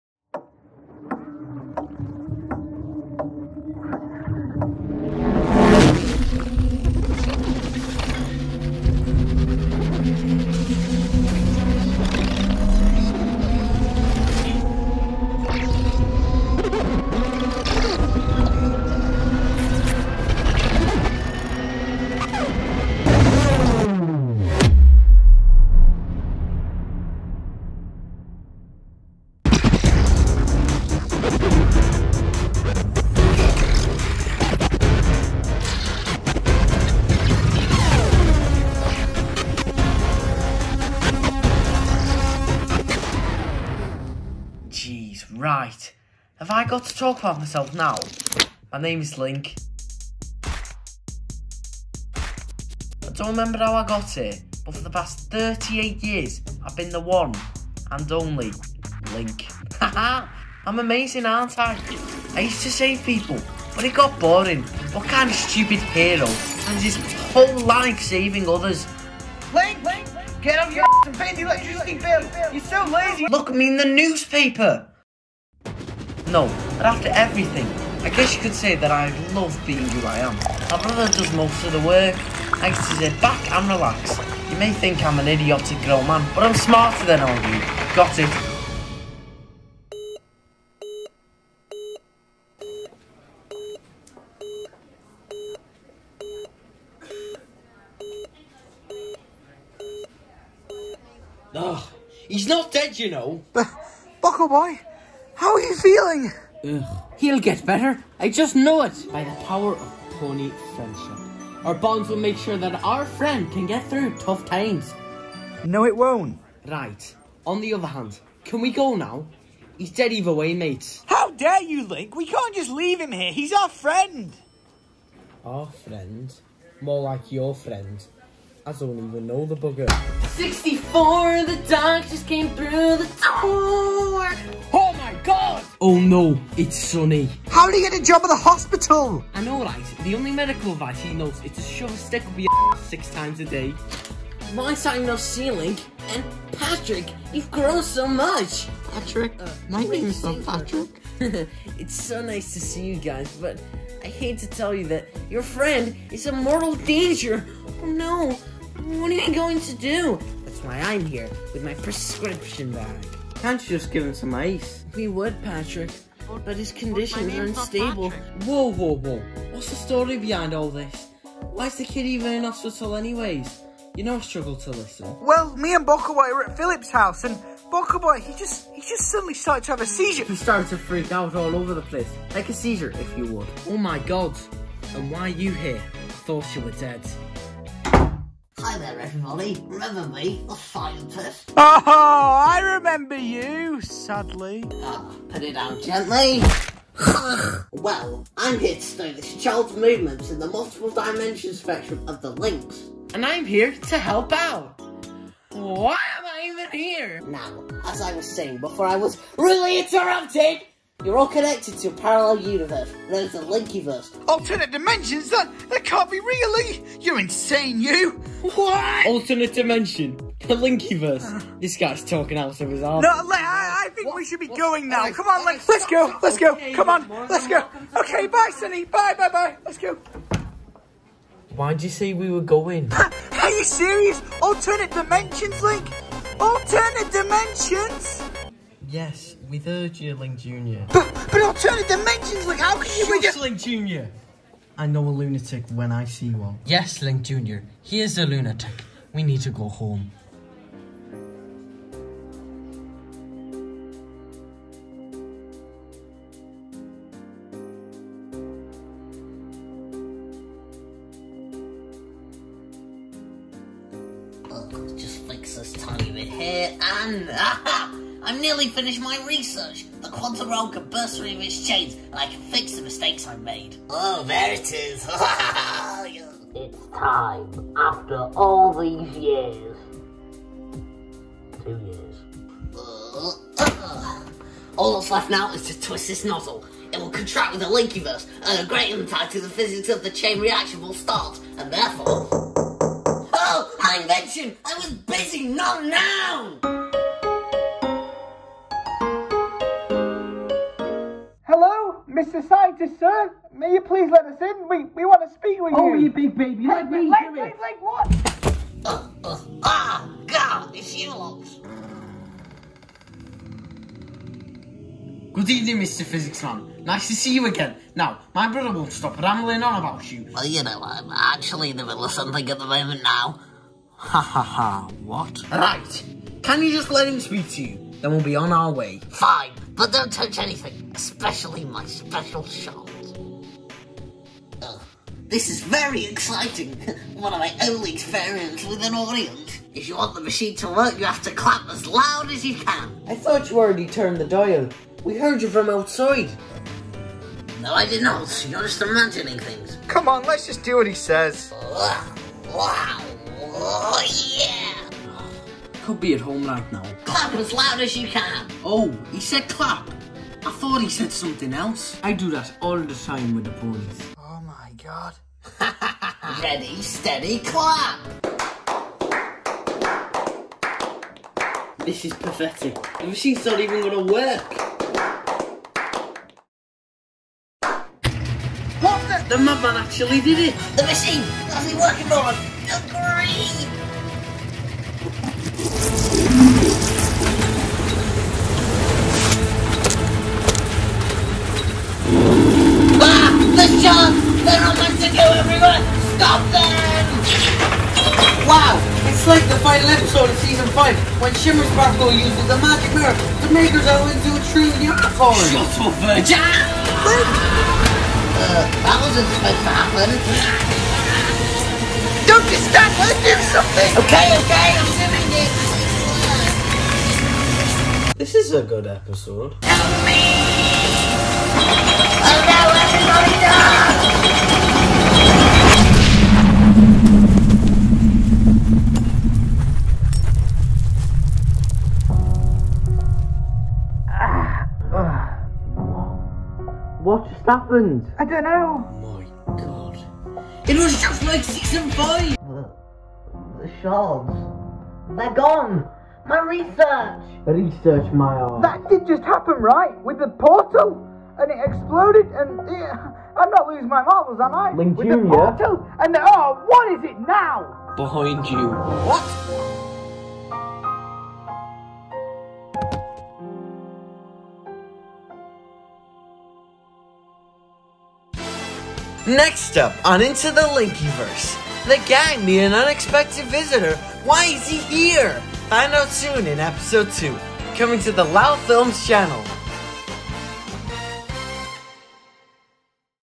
LaL Zelda Plush - Into the Linki-Verse | Episode 1
When brothers Link and Link Jr come across something they shouldn't have, they embark on an adventure that they will never forget!

Episode One:
The famous (and only) Scientist of Hyrule has created a new invention in order to fix something very important yet secretive. And when Bokkel Boi falls ill, his friends Link Jr and Phillip, along with Link, set out to try and save him...

Don't miss Episode 2!
Sunday 18 October